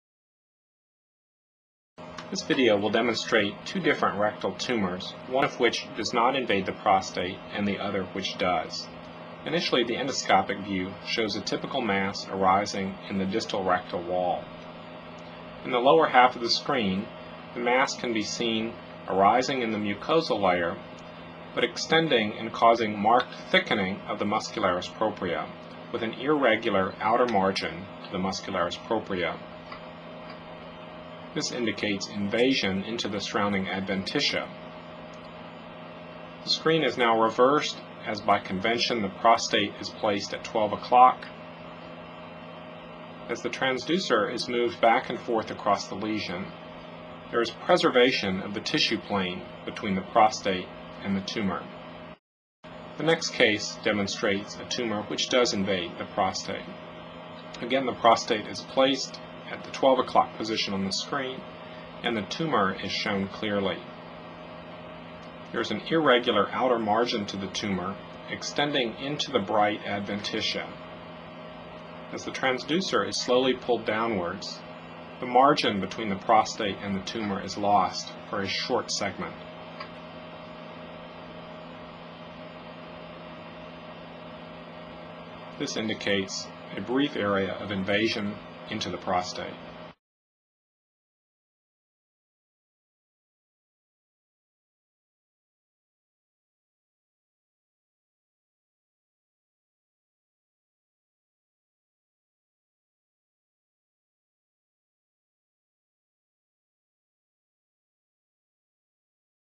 Rectal cancer
Dr. Michael Wallace discusses two different types of rectal tumors.